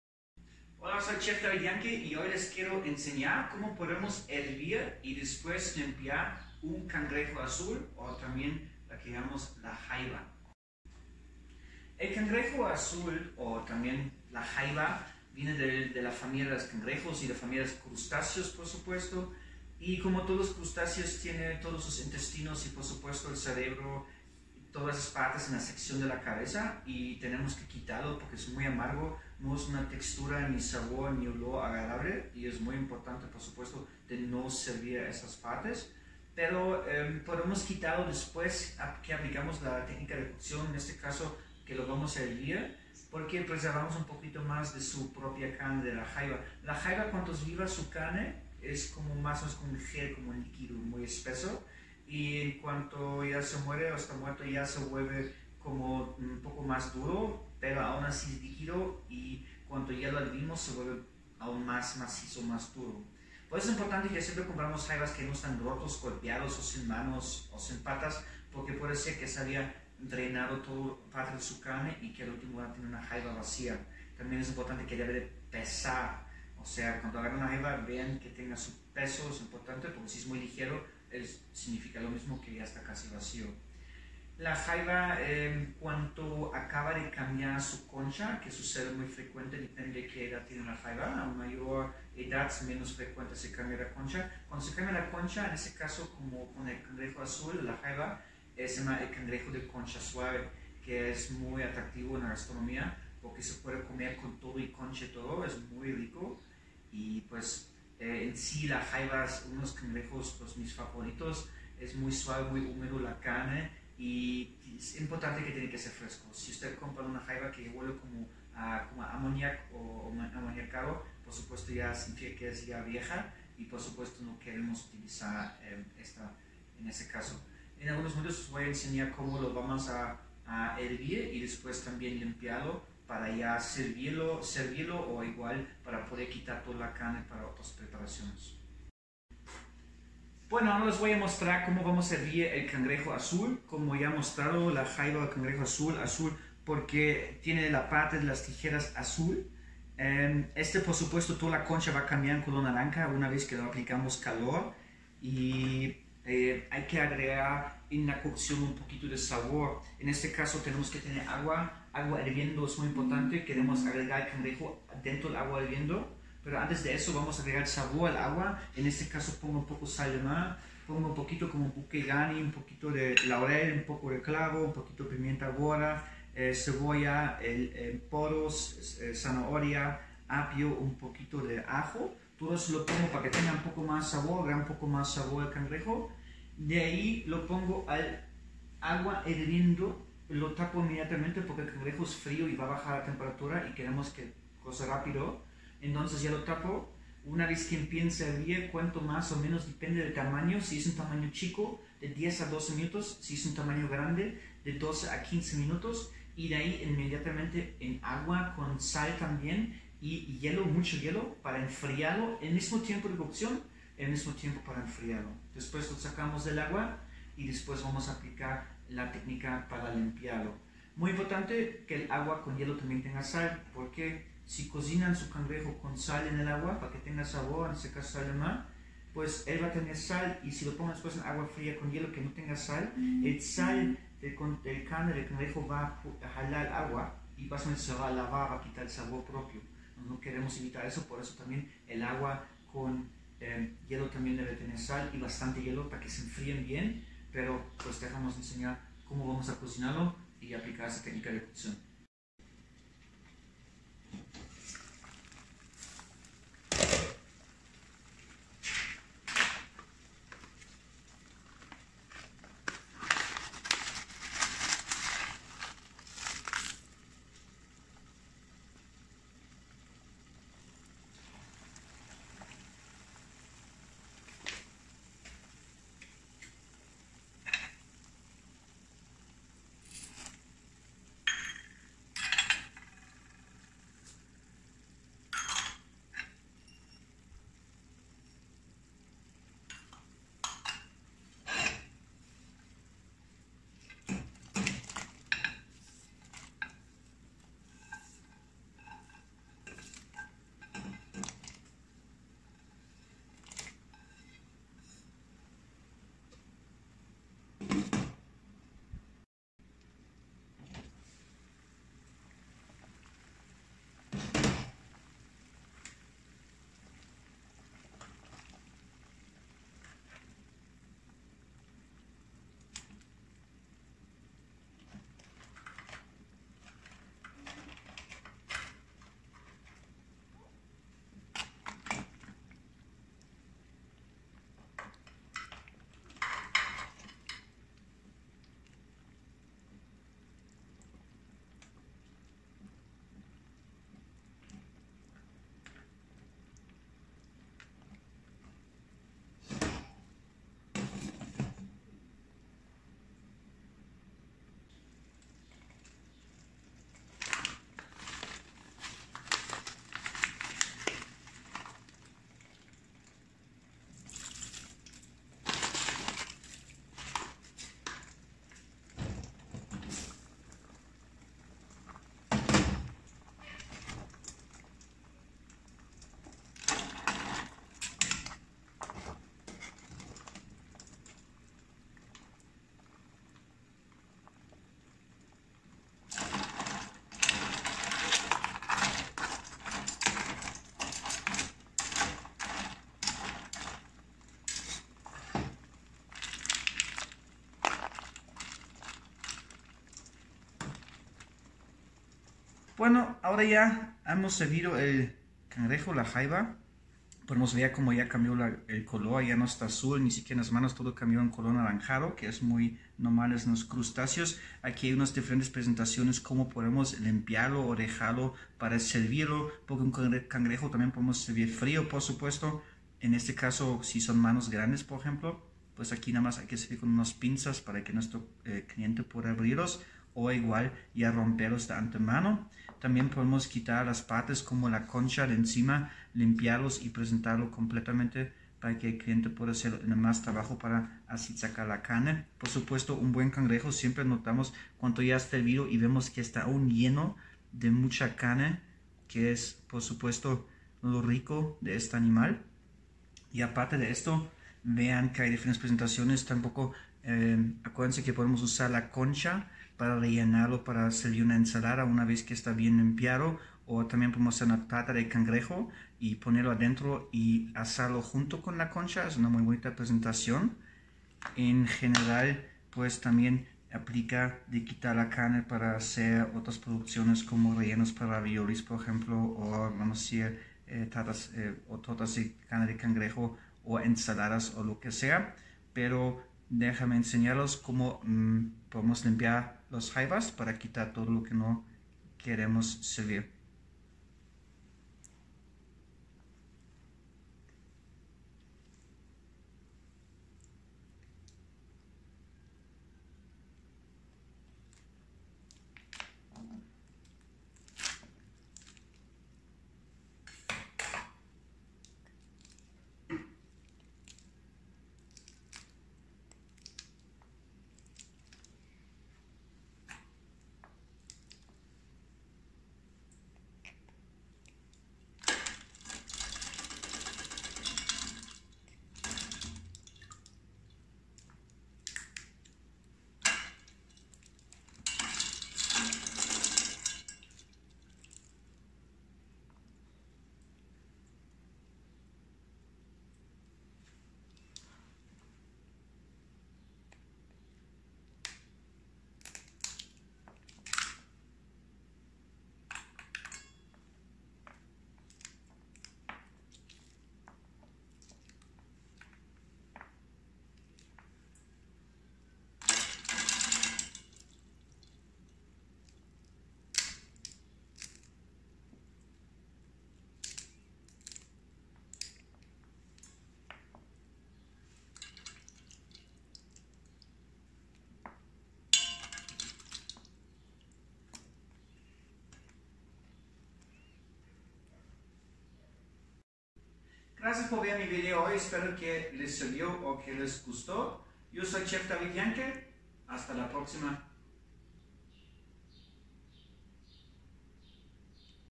Técnica culinaria, consejos sobre cómo cocinar y limpiar cangrejo azul o Jaiba Chef David Jahnke
Técnica culinaria, consejos y trucos sobre cómo cocinar y limpiar cangrejo azul o Jaiba fresco;
Permítanme compartir algunos datos y consejos, que podrían ayudarlo a aprender cómo cocinar y limpiar cangrejos azules frescos para servirlos fríos inmediatamente o para agregarle una técnica culinaria adicional.
Información general:
- El cangrejo azul es de la familia de los crustáceos y tiene todos sus órganos en la cabeza y un intestino que baja de la cabeza al cuento.
- Antes de servir un cangrejo azul, siempre queremos quitar los órganos, el cerebro y los intestinos, incluso si queremos usar las cáscaras para caldos, salsas o sopas.
- El cangrejo azul se puede preparar de muchas maneras, como a la parrilla, en guisos o sopas, salteado, embutido, cubierto con tempura, en ensaladas, ahumado, al vapor, como hamburguesas de cangrejo, hervidos y muchas otras formas.
- Cuando el cangrejo está vivo, su carne es como un líquido de textura de gel espeso que adquiere una textura más sólida una vez que el animal ha muerto, y más sólida una vez que cocina la carne.
- Asegúrese de que los cangrejos no estén dañados, rotos o sin patas o manos, ya que podrían haberse drenado por eso, si el cangrejo se siente a la luz, entonces probablemente esté vacío y no valga la pena comprarlo
- Cuando un cangrejo azul es viejo, huele a amoníaco y si un cangrejo azul huele mal, nunca deberíamos usarlo
Cómo cocinar cangrejo azul fresco:
Ingredientes:
5 l de agua
1 kg de hielo
30 g de sal marina
100 g de puerro
100 g de zanahorias
100 g de apio
100 g de cebolla
20 ajos
1 g de laurel
2 g de semillas de pimiento
1 g de clavo
Instrucciones: Mezcle 10 g de sal marina con el hielo y un litro de agua y tenga listo un lado. Pon todos los demás ingredientes a hervir juntos y una vez que esté hirviendo agrega el Cangrejo Azul y cubre la olla. Deje los cangrejos pequeños durante 1 minuto y los trozos más grandes durante 12 a 15 minutos. Del agua hirviendo, colóquese de inmediato en agua helada por el mismo tiempo, para enfriar los completamente.
Cómo limpiar el cangrejo azul cocido:
Gira el cangrejo sobre su espalda y levanta la historia para romper la concha completa del cangrejo azul. Ahora quítese las tripas y el cerebro y lave cuidadosamente el cangrejo azul, para lavar todas las partes de los intestinos y las tripas, pero no la carne.
Consejos adicionales:
Asegure su tabla de cortar con una toalla húmeda
Pregúntele a su vendedor de dónde viene Crab y cuánto tiempo ha pasado desde que salieron del agua, o búsquelo en el paquete si lo compra empacado para asegurarse de comprar la opción más fresca
Siempre compre cangrejo fresco (si es posible), será de mejor calidad
Trabaja rápido contigo Cangrejo, no los mantengas demasiado tiempo a temperatura ambiente
Nunca desperdicie alimentos, recuerde que las conchas y la cabeza se pueden usar para otras preparaciones, solo lávalas bien con agua fría
Cuando compres cangrejo azul fresco con cáscara, para pelarlos después de sacar la carne, considera que una vez que la pelaste y la limpiaste, perdiste un 50% - 60%, es importante calcular el peso bruto que necesitarás y el costo de su producto neto que necesita para la preparación de sus alimentos
Cuando sirves un cangrejo azul con cáscara, debes servirlo con una herramienta para abrir las cáscaras